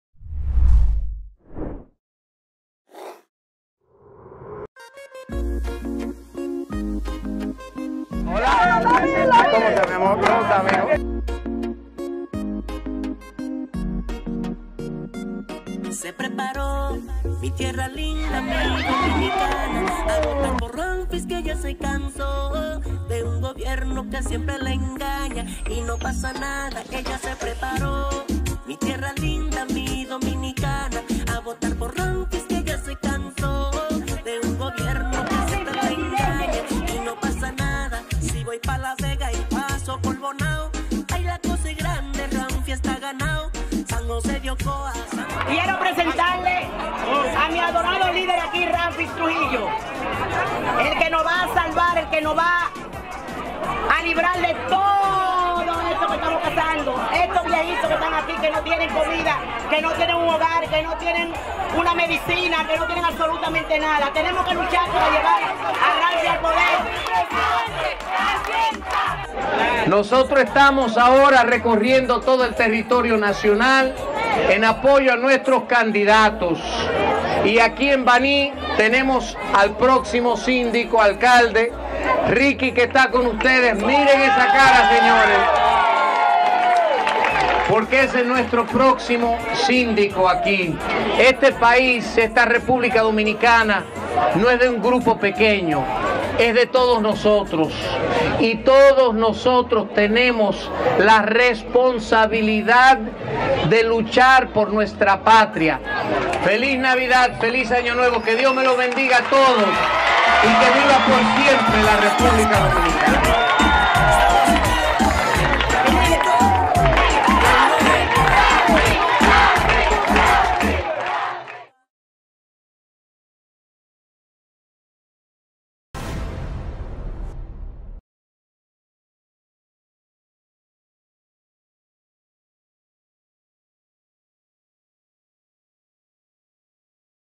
Ramfis Domínguez Trujillo encuentro con candidatos y el Movimiento Salones con Ramfis, en Baní
Cuenta oficial del candidato presidencial PNVC 2020 Ramfis Domínguez Trujillo. Dominicano🇩🇴 | Padre de tres hijas | Creyente fiel en Dios | Empresario | Demócrata | Patriota.